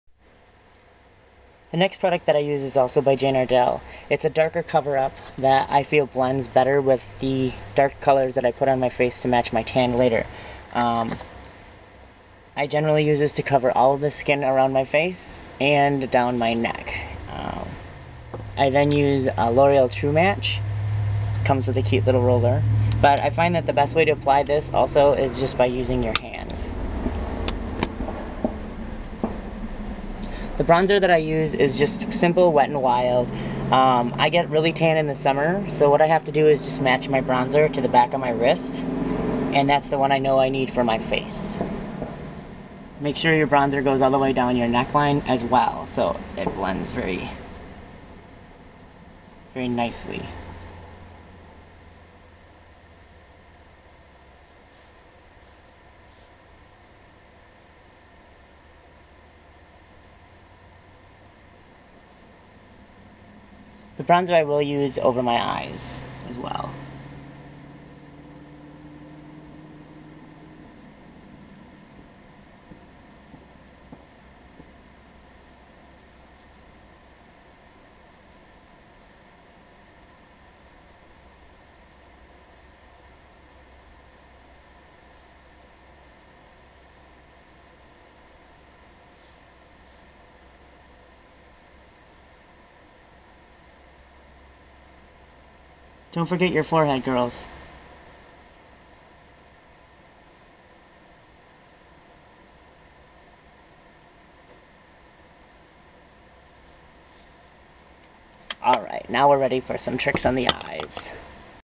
Diamond Sin-Sation Make-Up tutorial step 2. Covering blemishes, and adding your Bronzer
After applying your base foundation its important to find a bronzer thats matches your skin tone, and blends well with your facial features. Tune in to find out the details.